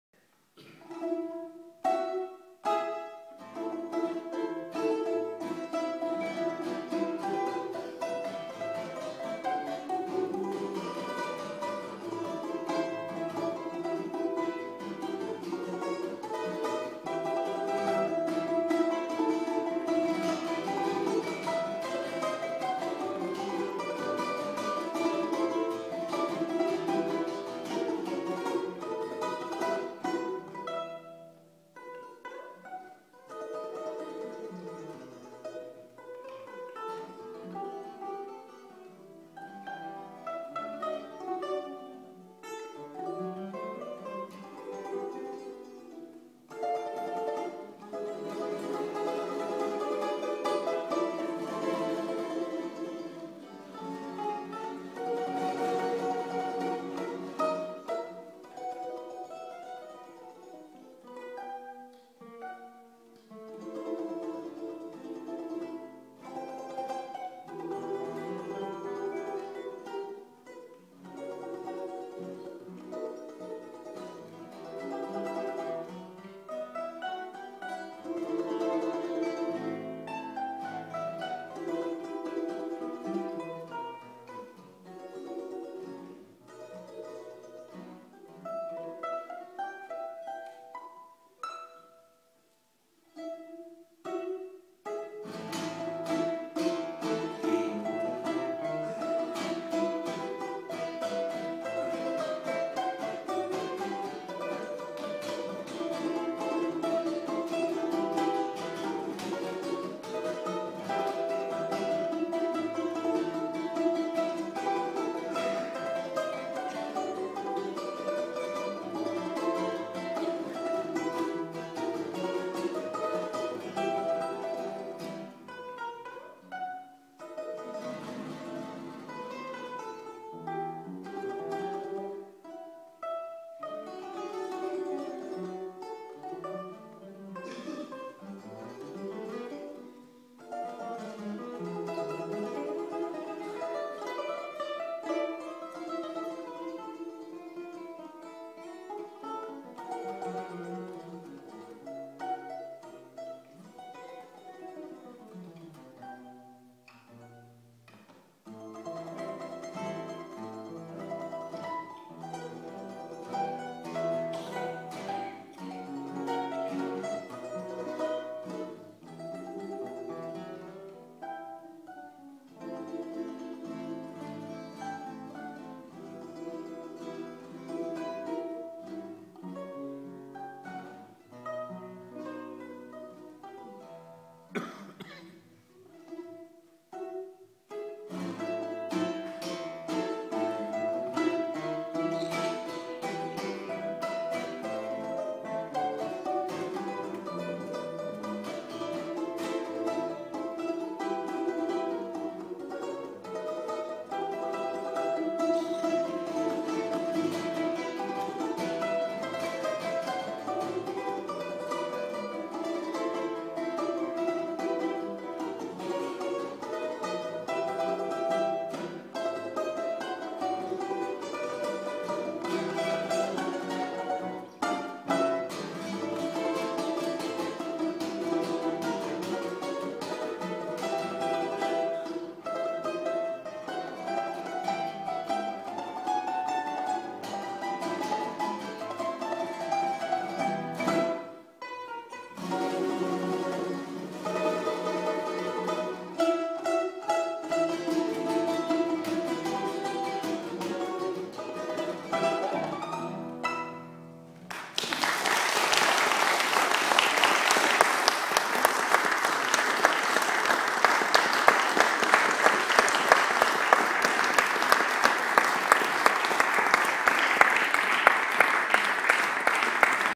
JXC & John Holden - Muzika Muzikantov 2
JXC & John Holden Performing Russian Classical and Folk Music in Mullion Churchtown on the 20/3/13 - Muzika Muzikantov - International Balalaika Duo.